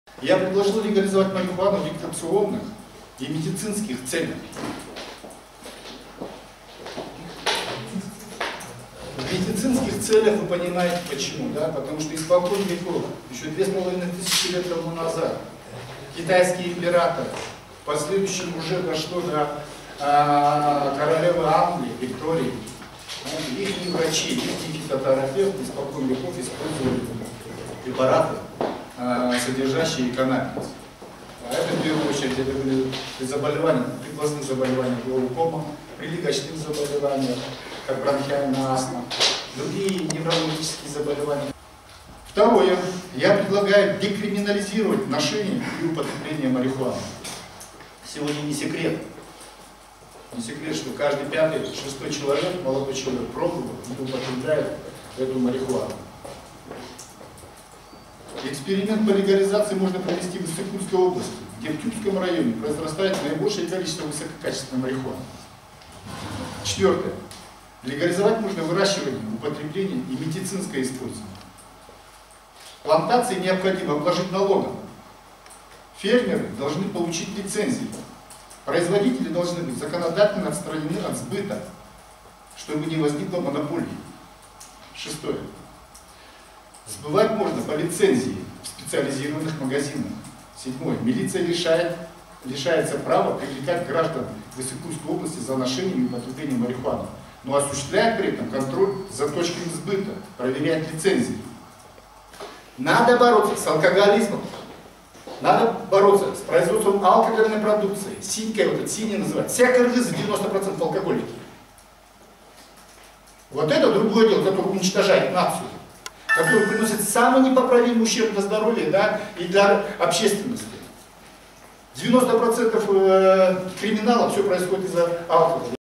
Назаралиев предлагает легализовать марихуану в Кыргызстане
Доктор медицинских наук Женишбек Назаралиев рассказывает о том, почему, по его мнению, необходимо легализовать марихуану в Кыргызстане. Легализацию он предлагает начать с Иссык-Кульской области.